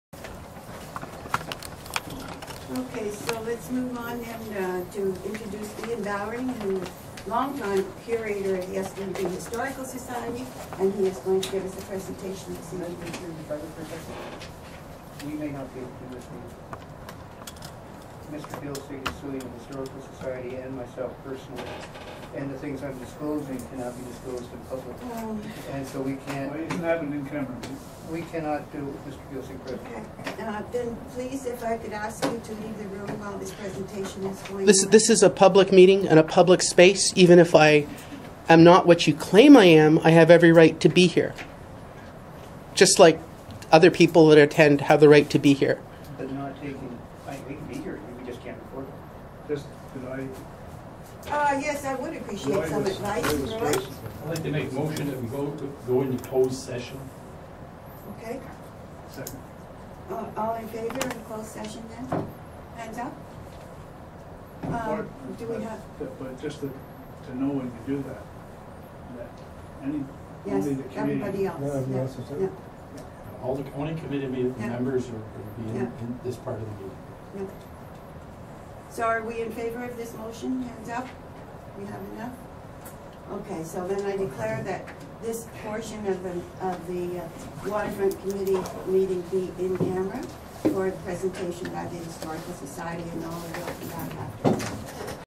Waterfront Committee in Cornwall Goes into Closed Session
Waterfront Committee   in Cornwall Goes into Closed Session after complaint from Ian Bowering  May 15, 2014